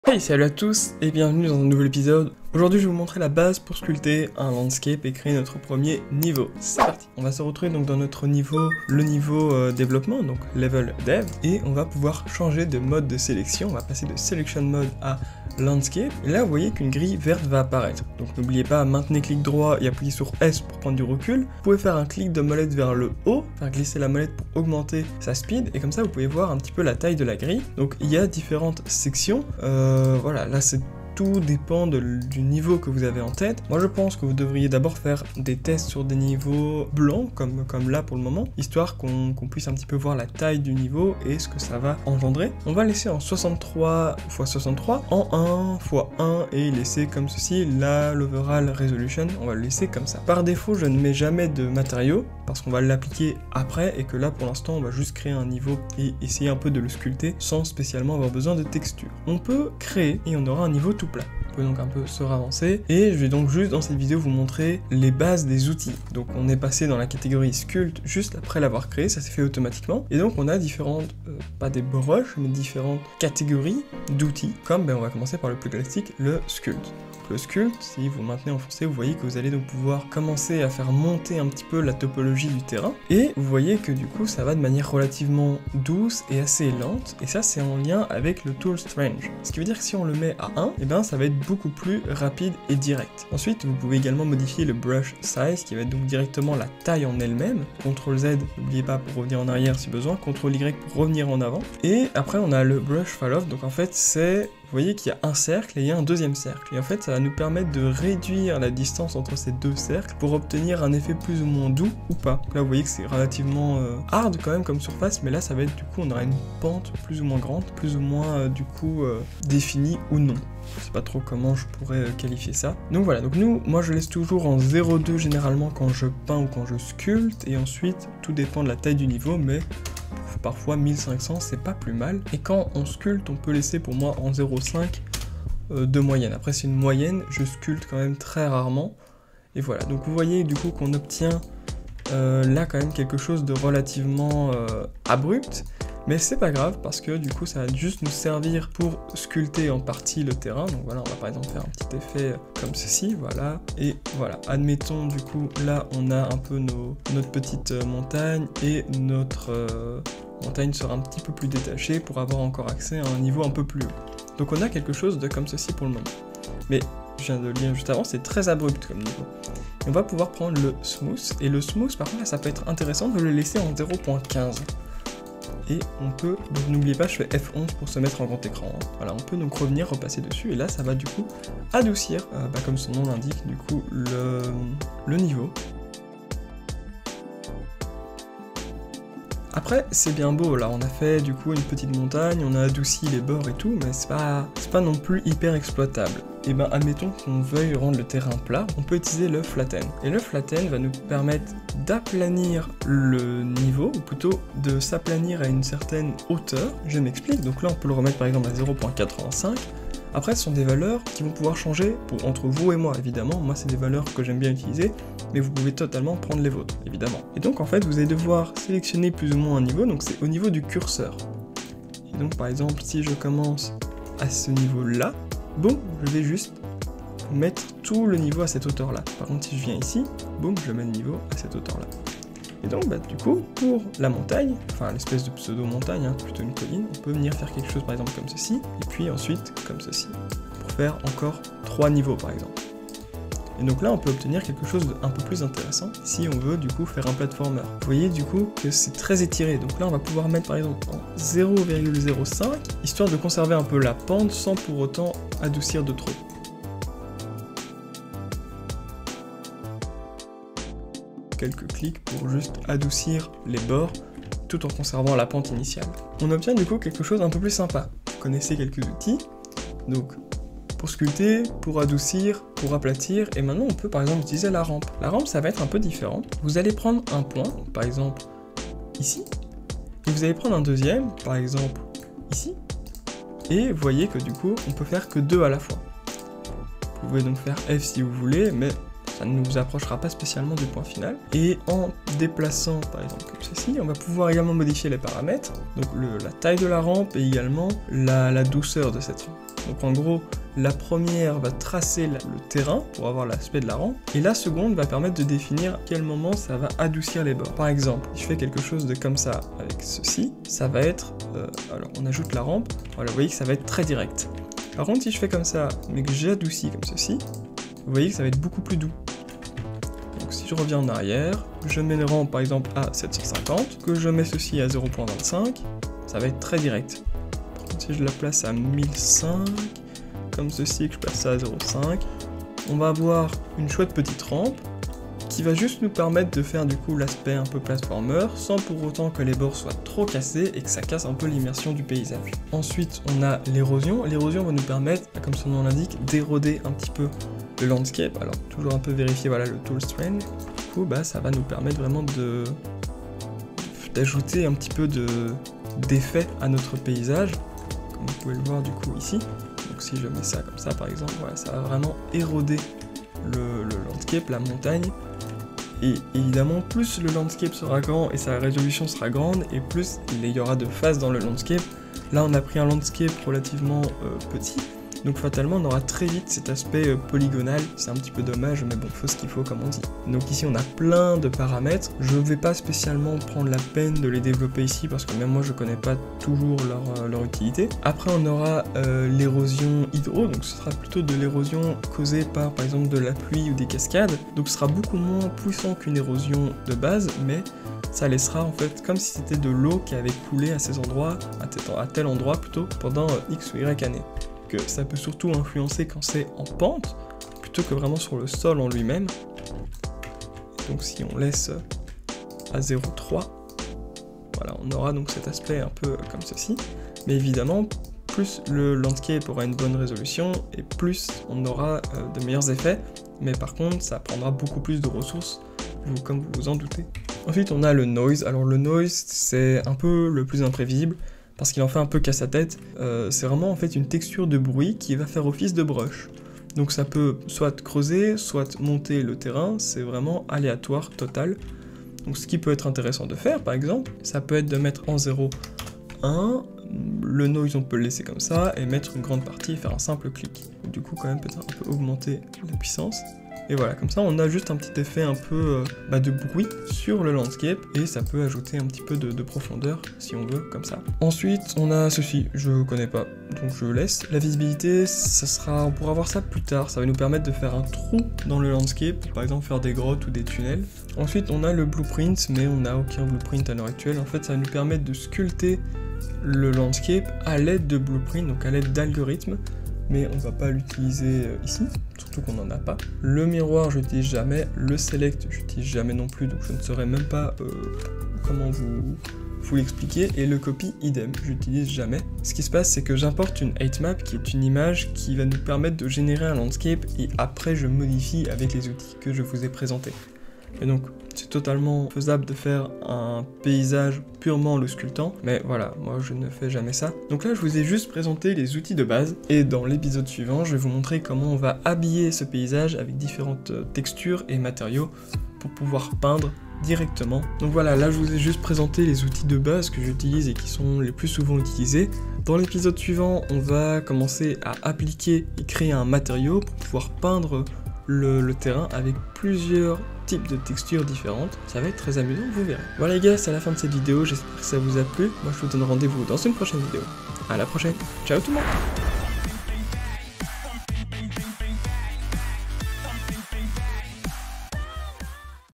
Créez un Platformer en 30 Jours sur UNREAL ENGINE 5 - Épisode 3 : Création d'un landscape
Bienvenue sur ma chaîne dédiée au développement de jeux vidéo avec Unreal Engine !

Dans cette formation, je vous guide pas à pas pour créer un platformer en seulement 30 jours. Aujourd'hui, nous allons voir les outils les plus utilisés pour la création d'un landscape dans Unreal Engine 5. Bonne vidéo !

🚀 Version du moteur : Unreal Engine 5.4

*{Time code}*
 00:00 - Intro
00:10 - Create Landscape
01:06 - Sculpt Tool
02:48 - Smooth Tool
03:20 - Flatten Tool
04:30 - Ramp Tool
05:40 - Erosion Tool
07:08 - Noise Tool
08:58 - Outro